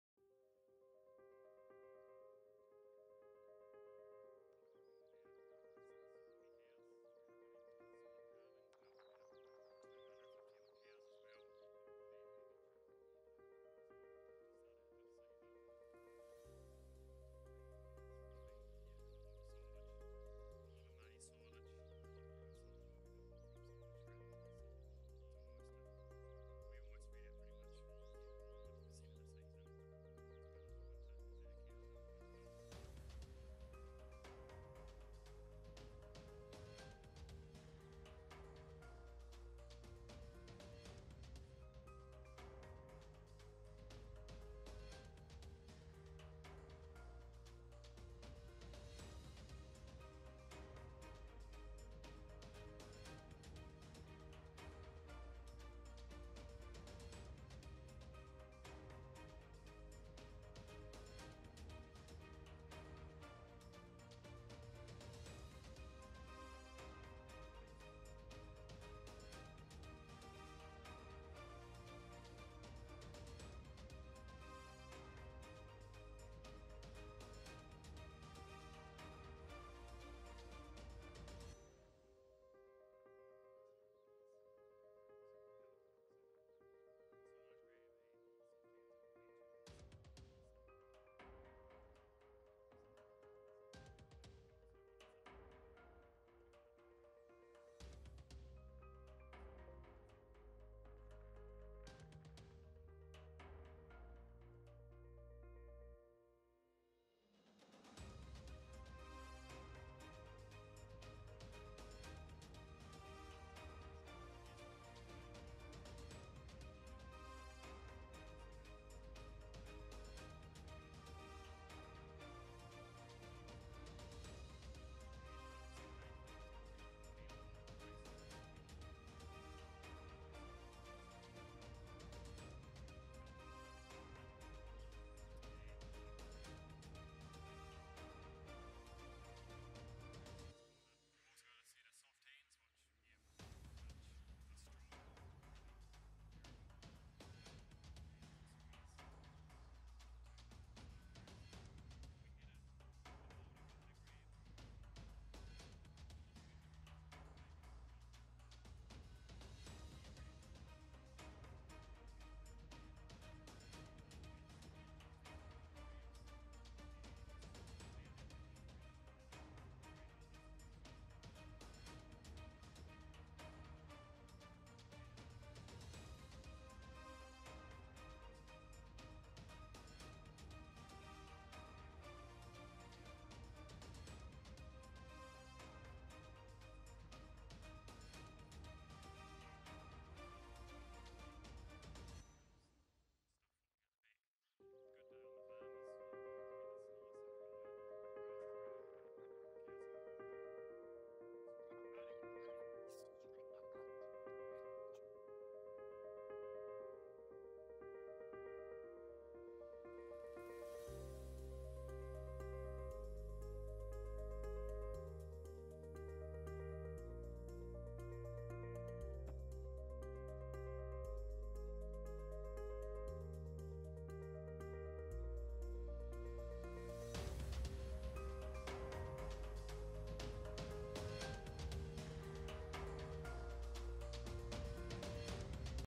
Will Bailey | Loader Attachments Review
Will Bailey let's us know his review on a range of Rata Loader Attachments. 'Strong, Reliable and Durable'. Find out what he has to say on the Bale Grabs, Bale Forks, Silage Sabre, Hay Forks and Combo Set.